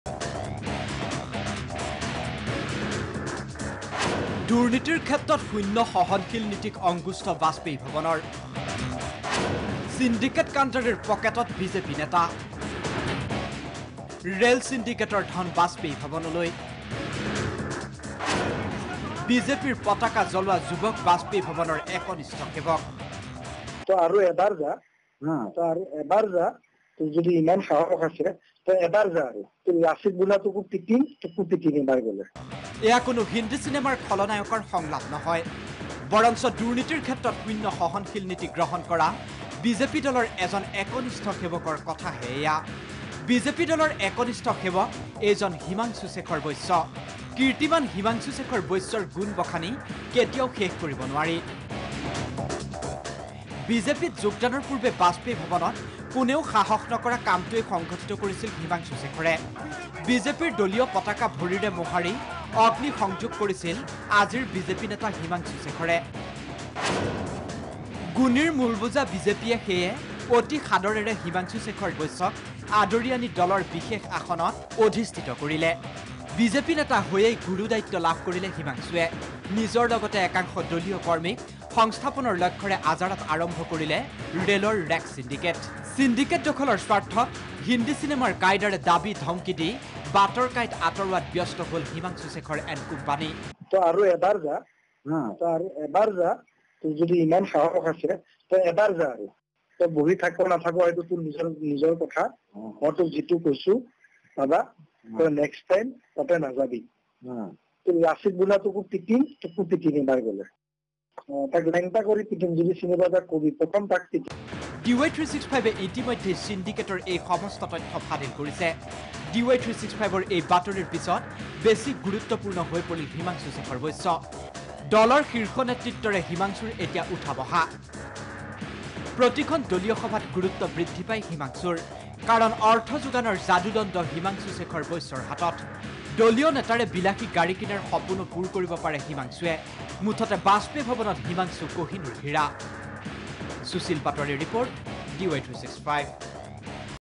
বিজেপিত কদৰ বাঢ়িছে ছিণ্ডিকেট চক্ৰ || BJP Syndicate || Rail Syndicate
বিজেপিত কদৰ বাঢ়িছে ছিণ্ডিকেট চক্ৰ। এসময়ত বিজেপি পতাকা ভৰিৰে মোহাৰি অগ্নি সংযোগ কৰা ছিণ্ডিকেট কাণ্ডাৰীক লৈ বিশেষ ব্যস্ত বাজপেয়ী ভৱন।
Watch to know more: